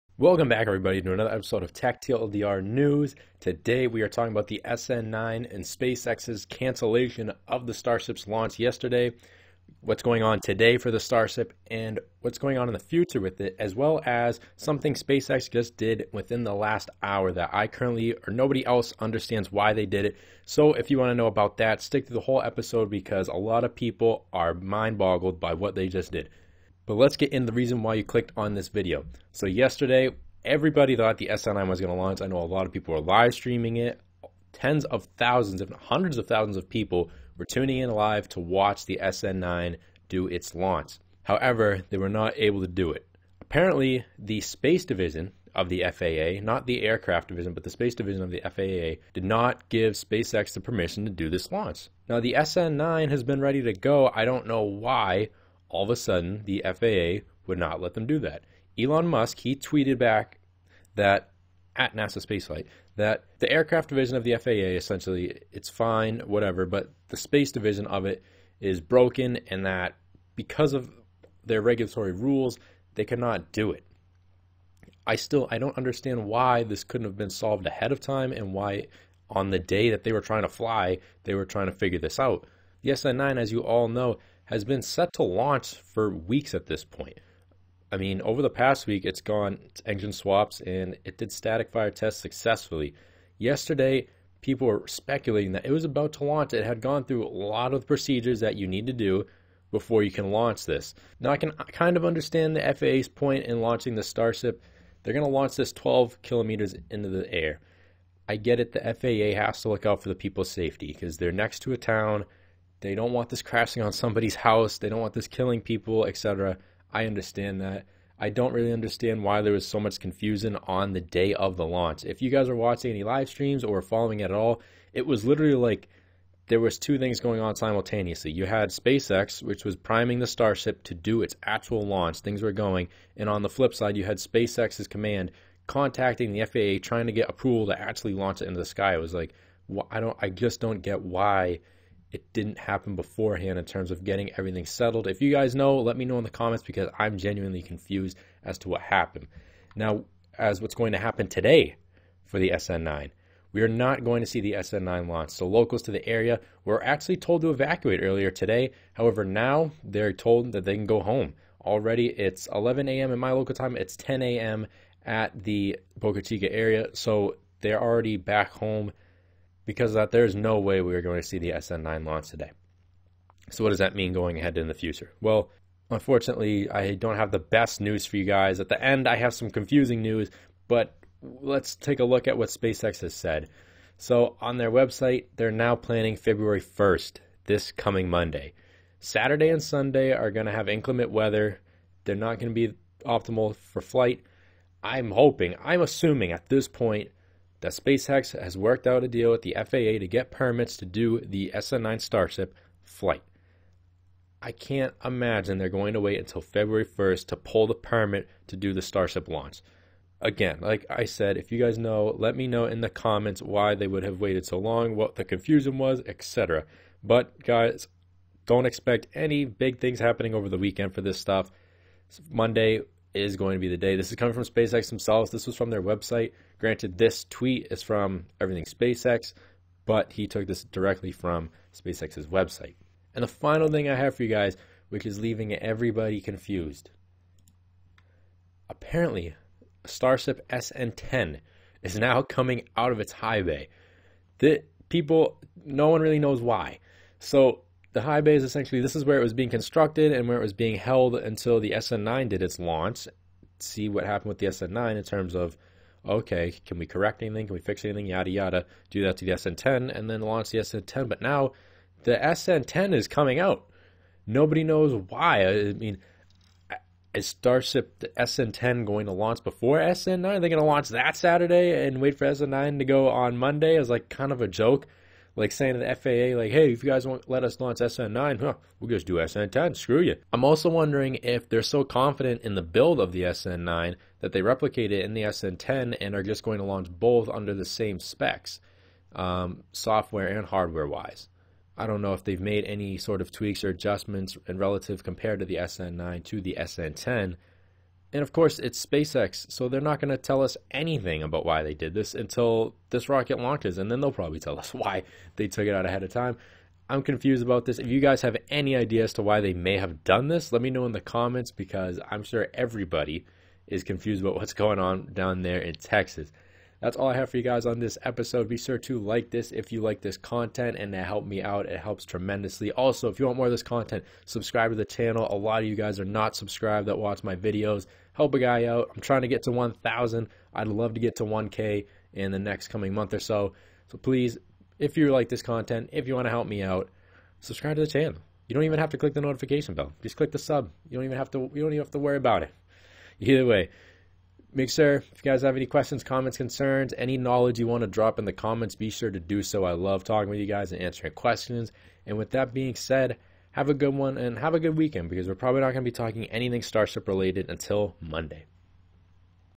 Elon Musk Tweets About Possible SN9 Launch [1/29] | SpaceX News
SpaceX's Elon Musk has finally shed some light on the issues delaying the launch of the SN9 starship. Elon Musk took to twitter to give an update regarding the SN9. In todays episode, we talk about the SN9 launch date as well as other news regarding SpaceX.